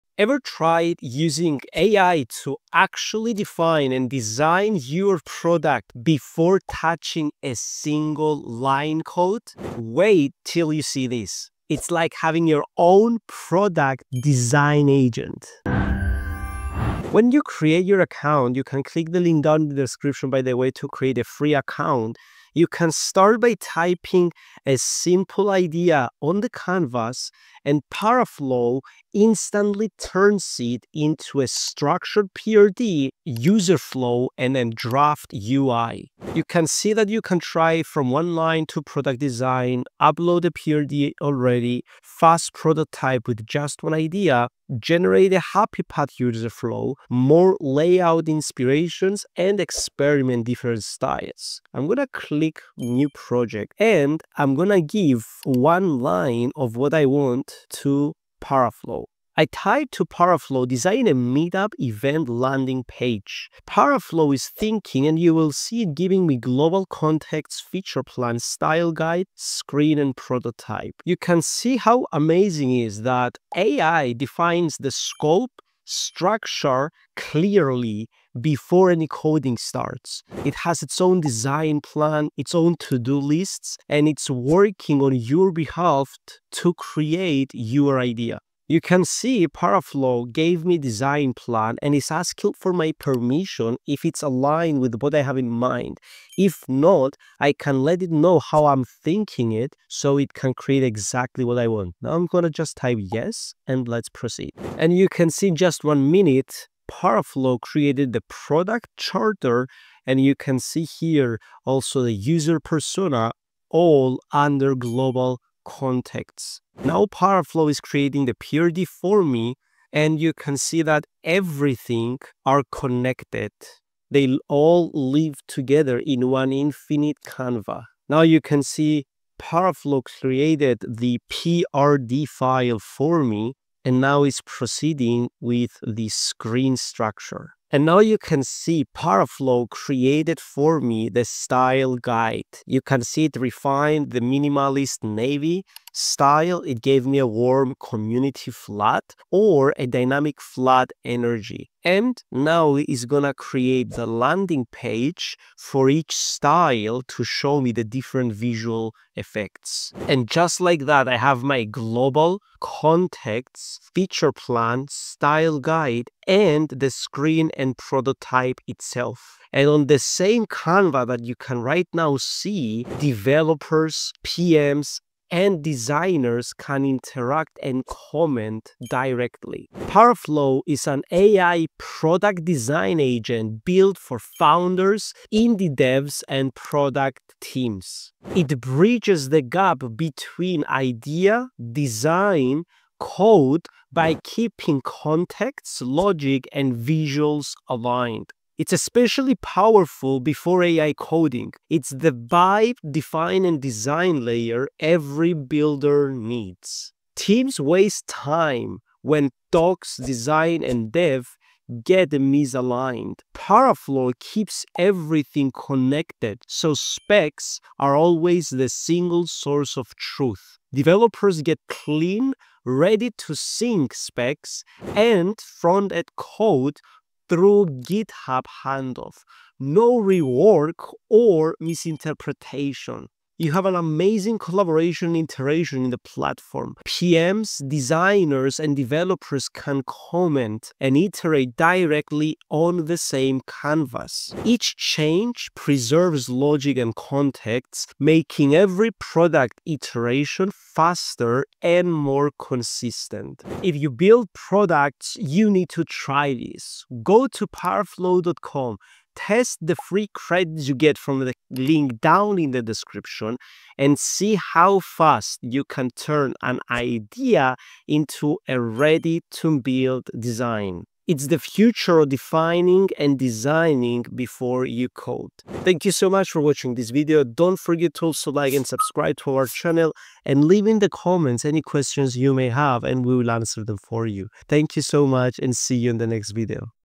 AI Tools To Make Money? Turn Simple Ideas Into Real Businesses ? (Honest Review)
AI Tools To Make Money ? Innovative Product Design Software | Transform Ideas Into Prototypes ?

💥 Rank Your Video at the TOP 🚀 Get More Visibility with YTRanker!

Looking for innovative AI tools that help you create, build, and organize your ideas efficiently? In this video, we explore Paraflow — an AI-powered product design agent that assists in turning ideas into structured design outputs in a faster and more streamlined way.

In this video, you'll discover:
-How Paraflow can convert a single idea into a PRD, user flow, and draft UI structure.
Why Paraflow can be useful for founders, indie builders, and product teams who want faster alignment during product creation.
How Paraflow enables smoother collaboration between PMs, designers, and developers through a unified visual workspace.
How AI assists in simplifying product development workflows and reducing misalignment during decision making.
How Paraflow can help generate prototypes, style direction, and layout structure in a short period of time.

Paraflow acts as a connector between idea, design, and build stages — so product logic, specs, and visual direction can stay organized in one place. Whether you are a small business owner, product manager, startup founder, or developer — this tool shows a new way to approach design and product planning using AI.

ai tools to make money ai productivity tools productivity with ai automation for small businesses ai-powered solutions

CHAPTERS:
00:00 - Power FLOW - Define, Design & Prototype Your Product
00:49 - Power FLOW - Generate User Flow & Style Guide
01:22 - Power FLOW - Get Your Design Plan & PRD
02:10 - Power FLOW - Create Your Product Prototype
02:49 - Power FLOW - Get Your Product Charter & User Persona
04:35 - Thank You For Watching - Don’t Forget To Like & Subscribe